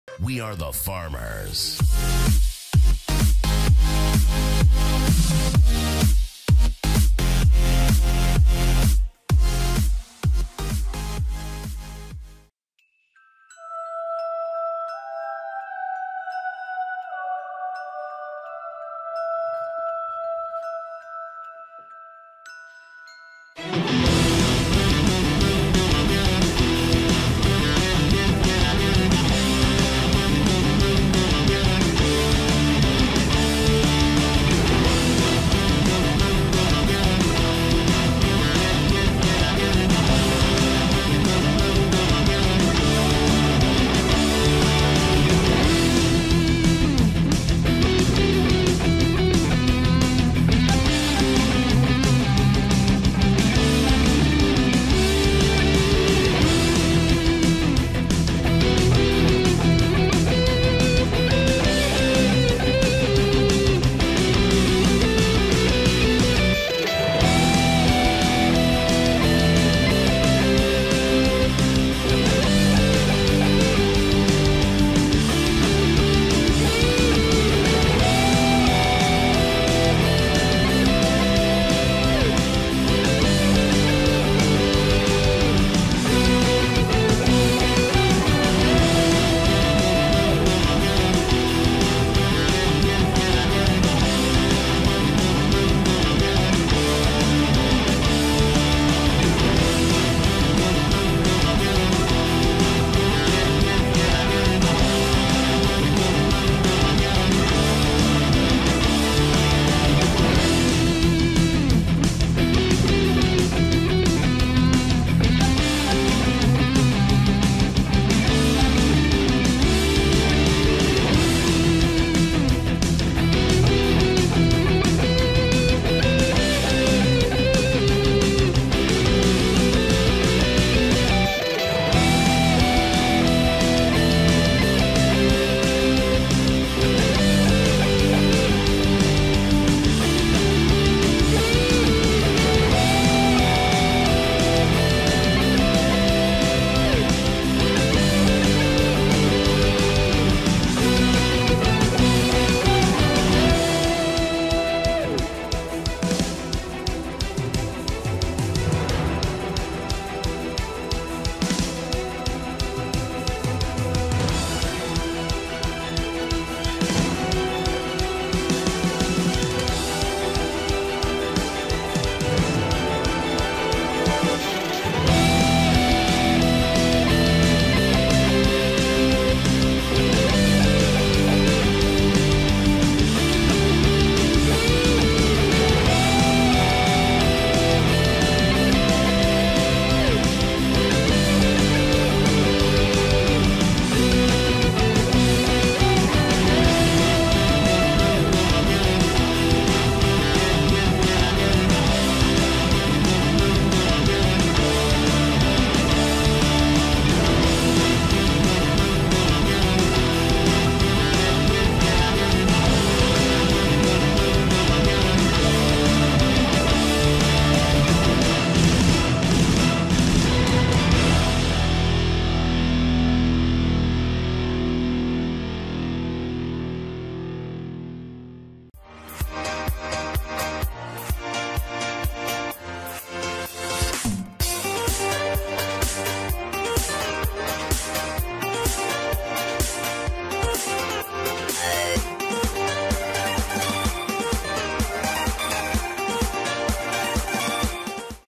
Schneeräumen 2015 ► 500 Abo Spezial !! ► Filmteam Oberpframmern
Hier ist nun endlich das lang ersehnte Video vom Schneeräumen, im Winter 2015. Dieses Jahr hatten wir sehr viel Schnee, hoffentlich schneit es noch ein, zwei mal gscheid :D.

Kameras:

► 2x Rollei Bullet 5s (Actioncam)

► 1x Panasonic Camcorder